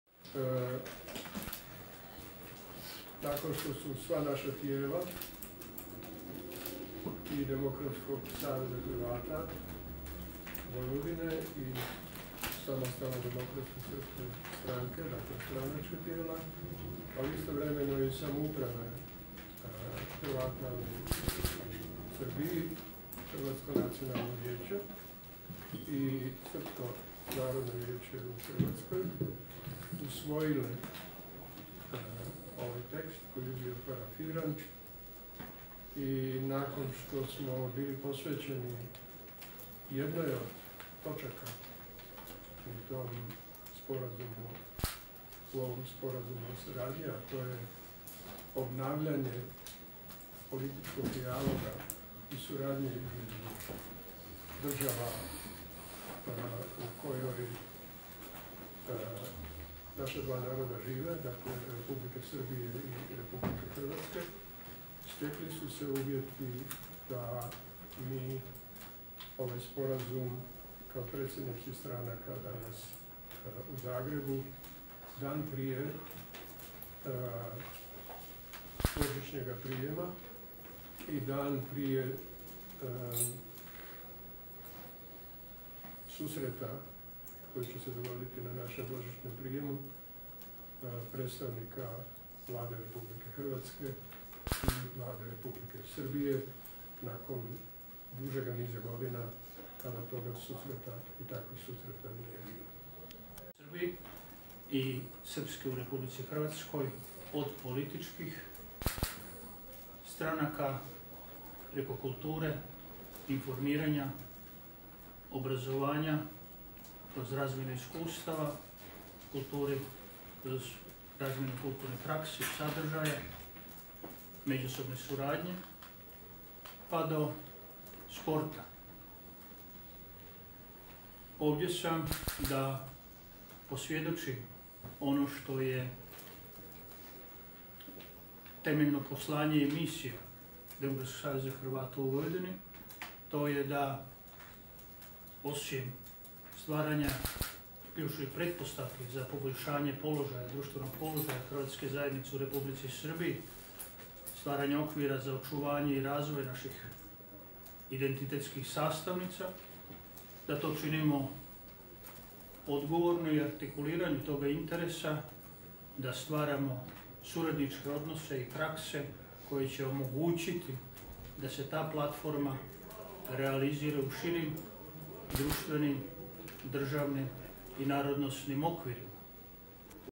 Pupovac: 'Suradnja je duga. Nismo slučajno ratificirali i danas potpisali dokument o našoj suradnji'
Predsjednici SDSS-a Milorad Pupovac i Demokratskog saveza Hrvata u Vojvodini (DSHV) Tomislav Žigmanov potpisali su danas Deklaraciju o suradnji Srba iz Hrvatske i Hrvata iz Srbije.